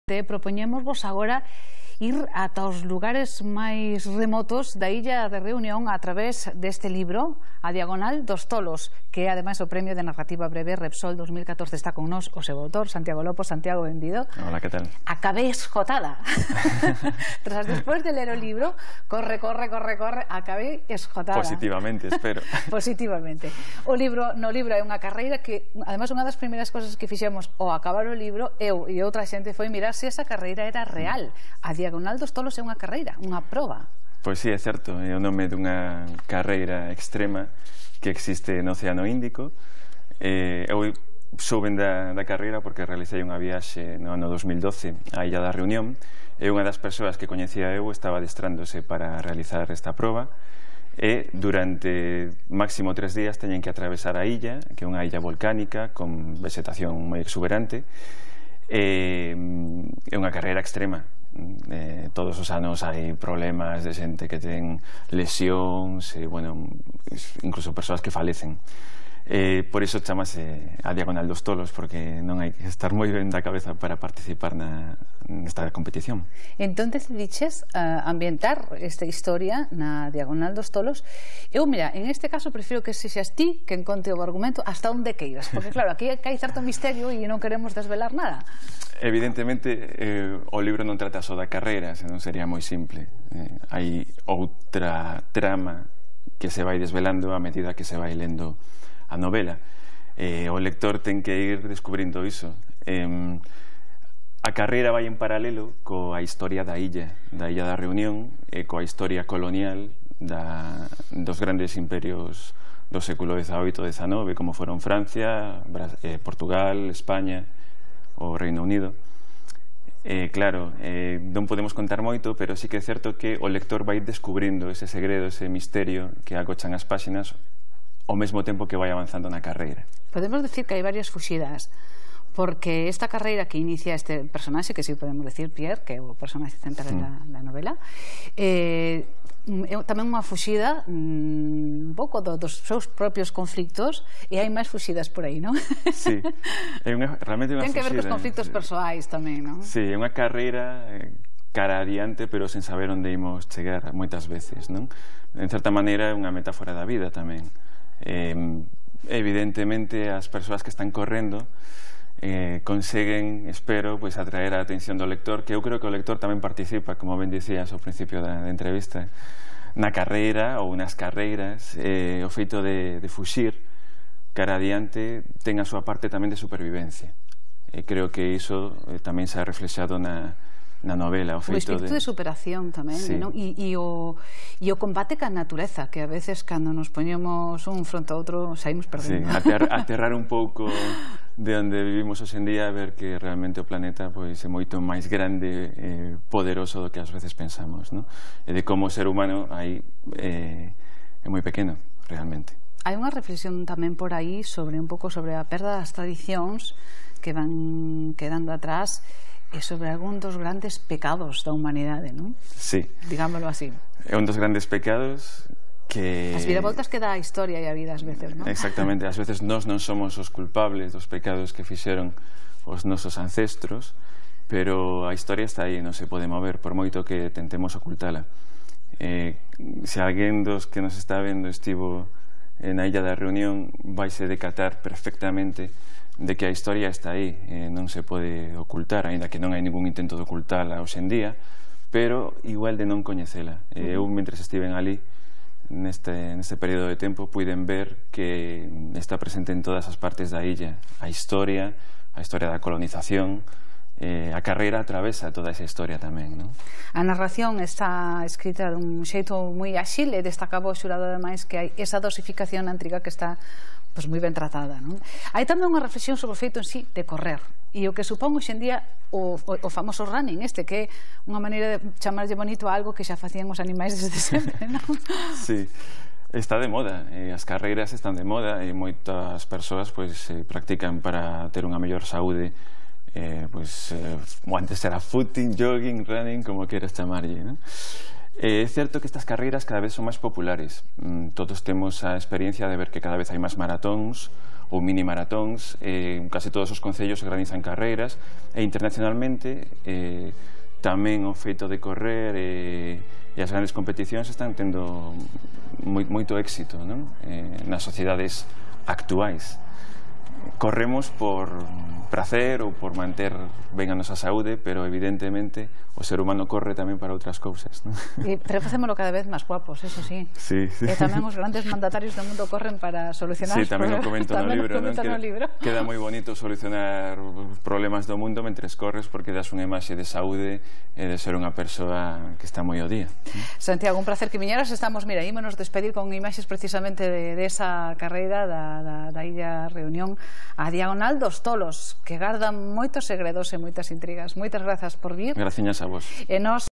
Entrevista a Santiago Lopo: "A Diagonal dos Tolos"
Entrevista a Santiago Lopo na TVG (2014) sobre a súa novela "A Diagonal dos Tolos"